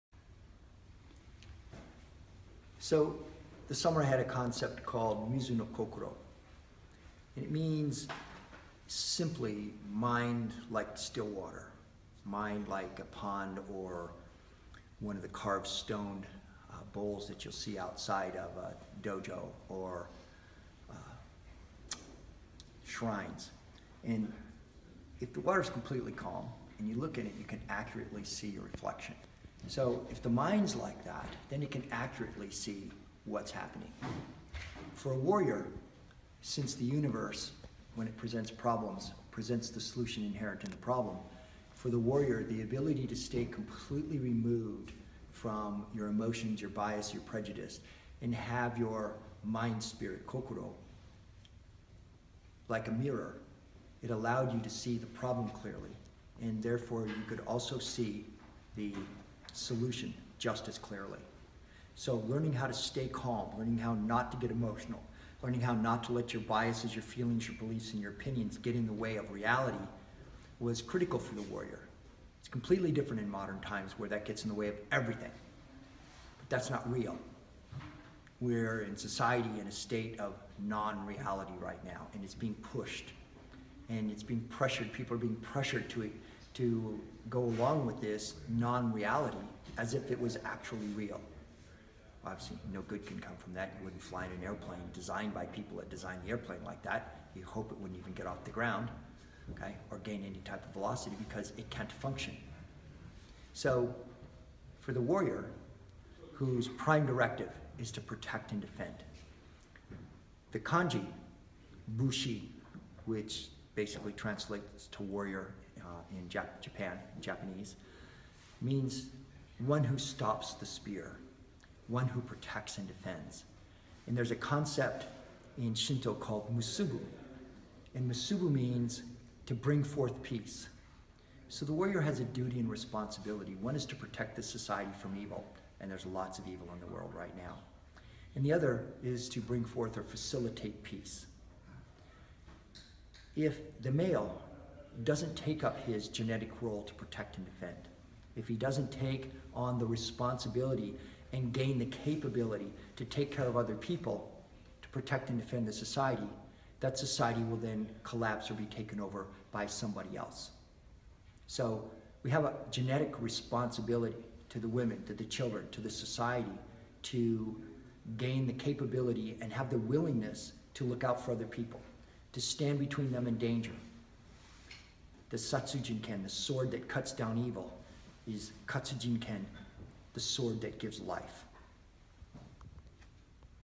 Mizo No Kokoro - James Williams Sensei
Taken in May 2016, but very relevant today. In this video, James Williams explains various concepts including Mizo No Kokoro and others of importance to understand the Samurai way. He also warns how a false reality is being pushed to society these days and talks about the duties and responsibilities of the warriors in defending society.